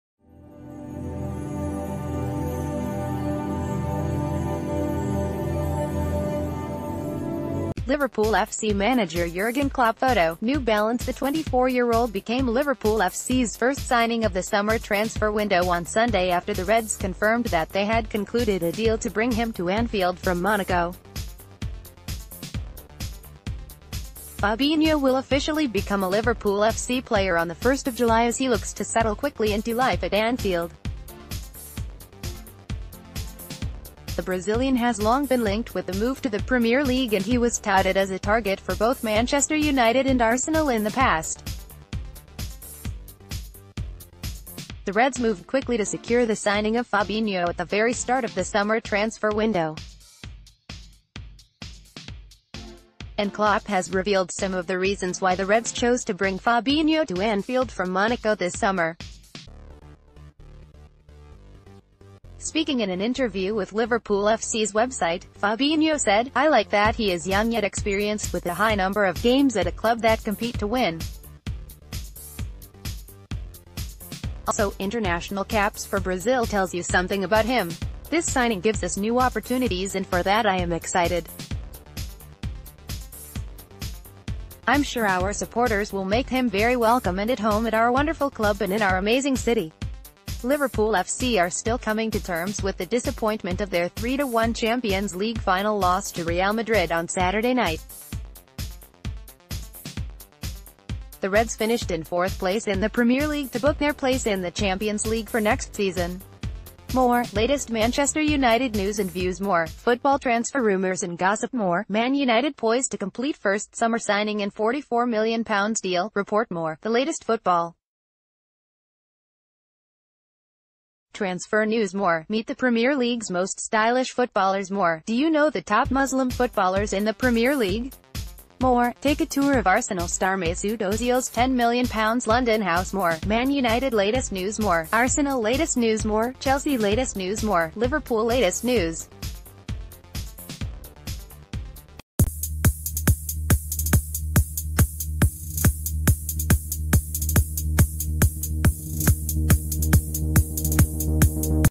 Breaking News | Jurgen Klopp explains Liverpool FC's first summer signing
Liverpool FC manager Jurgen Klopp Photo: New Balance The 24-year-old became Liverpool FC’s first signing of the summer transfer window on Sunday after the Reds confirmed that they had concluded a deal to bring him to Anfield from Monaco. Fabinho will officially become a Liverpool FC player on 1 July as he looks to settle quickly into life at Anfield. The Brazilian has long been linked with a move to the Premier League and he was touted as a target for both Manchester United and Arsenal in the pa...